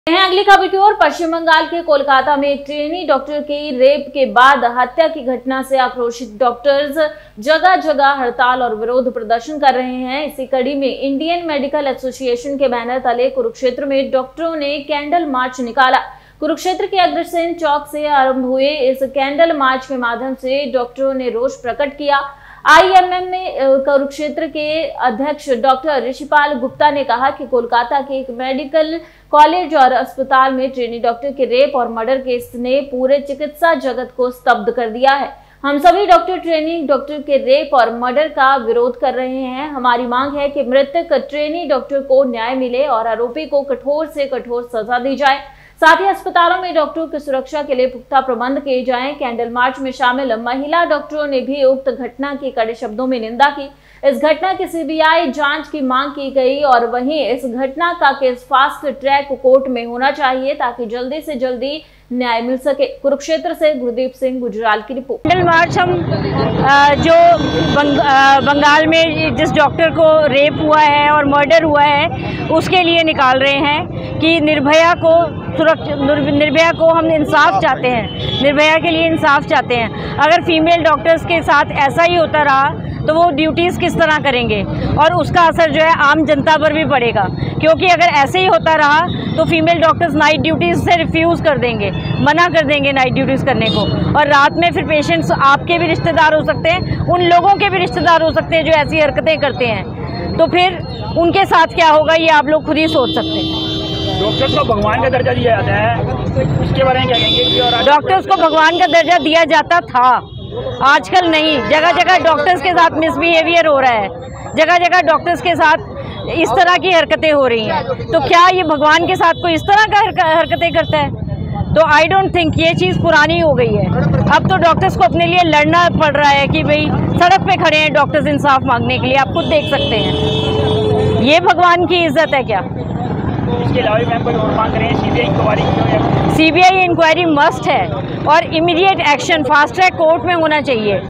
कुरूक्षेत्र : कोलकाता में डॉक्टर की रेप हत्या से आक्रोशित डॉक्टर्स ने कुरुक्षेत्र में
Raftaar Media